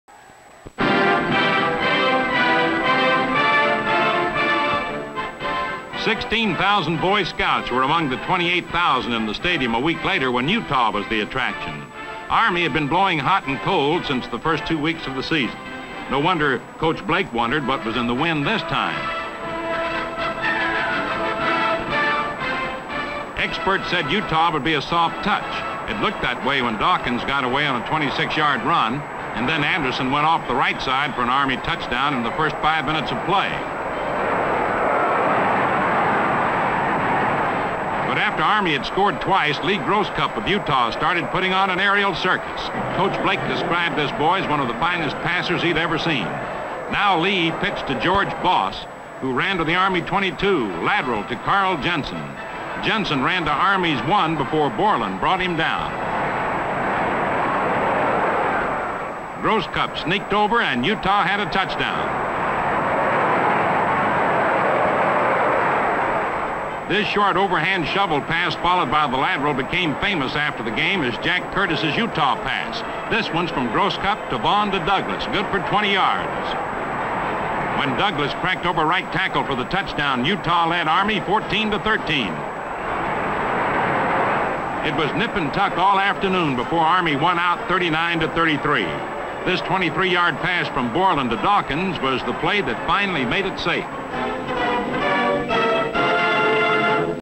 Throwback Thursday: 1957 vs. Utah
Take a look back at Army's win over Utah in 1957.